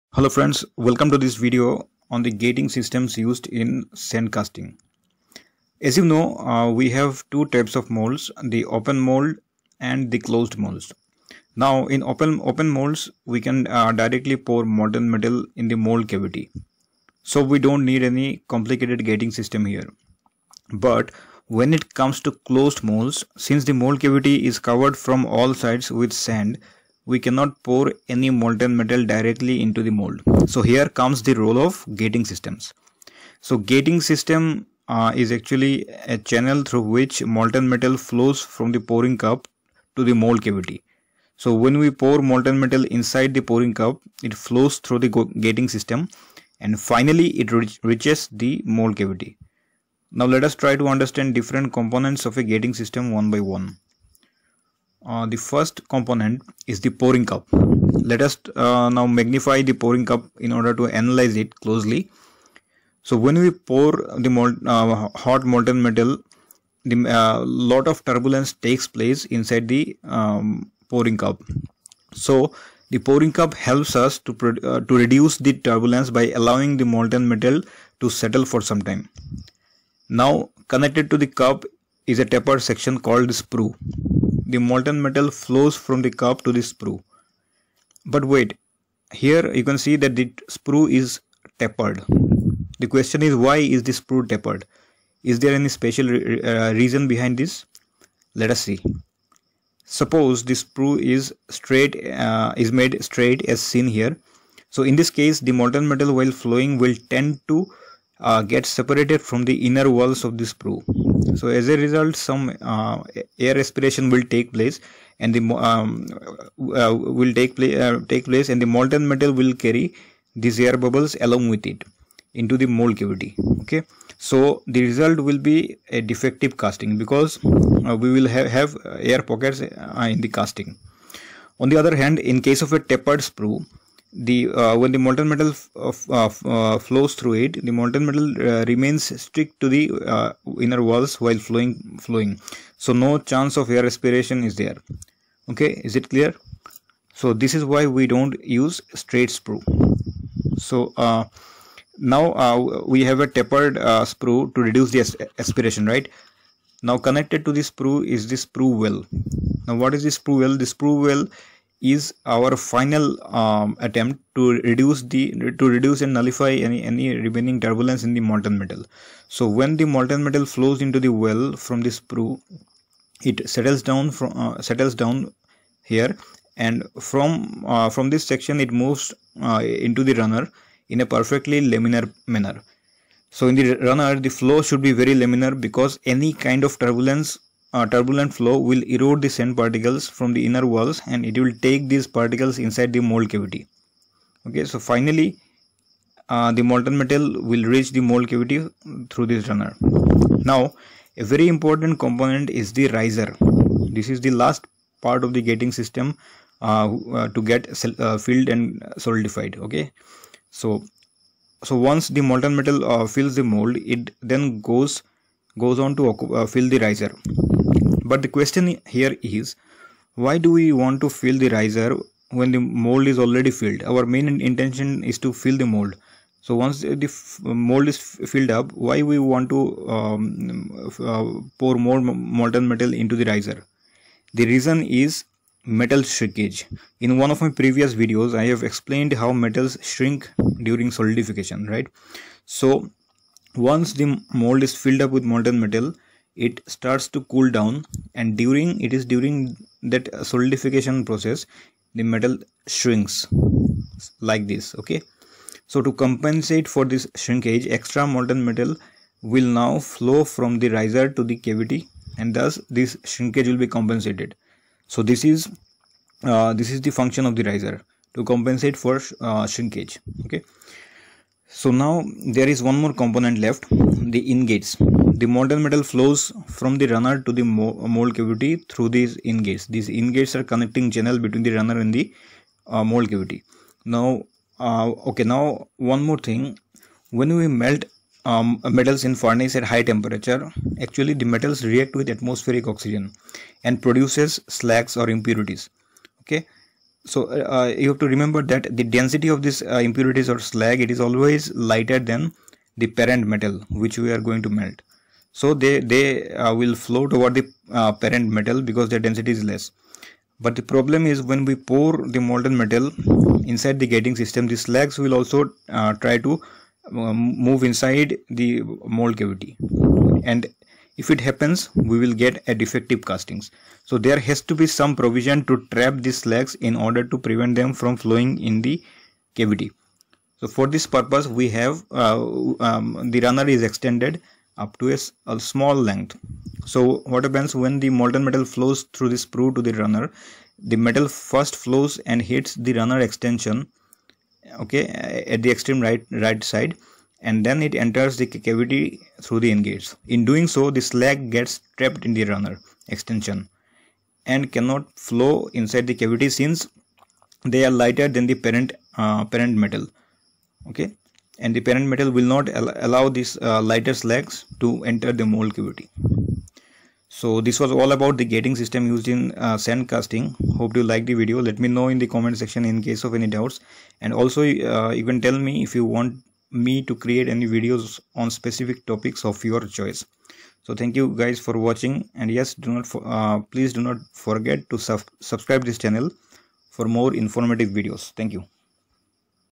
Gating System in Sand Casting - Explained Easily with Animation (Basics, Components and Design)
In this video, the gating systems used in metal casting processes have been discussed very easily with the help of simple animation.
The video starts with a short explanation of what is a gating system and why we need a gating system. Then it proceeds on to discuss different parts of gating system in details:
i) Pouring cup
ii) Sprue
iii) Sprue well
iv) Runner
v) Ingates
vi) Risers
 You can also browse through some other concepts of metal casting process by clicking the following links:

3) Properties and characteristics of molding sand used in sand casting